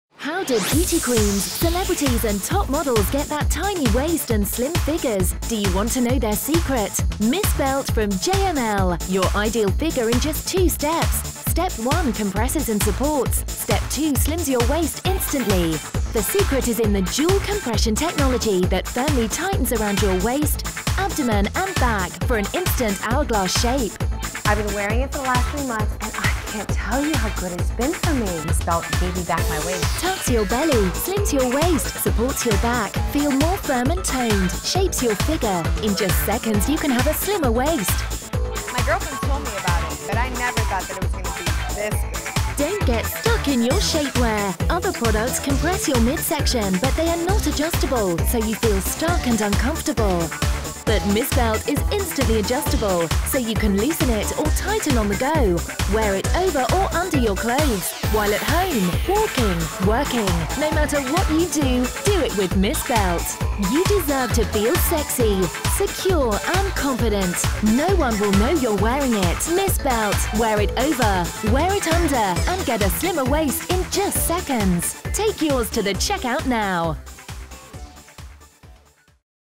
Miss Belt from JML
Have the figure you desire in an instant.

Now you can support your back, hold in your tummy and look two sizes slimmer with the amazing Miss Belt, for an instant hourglass figure.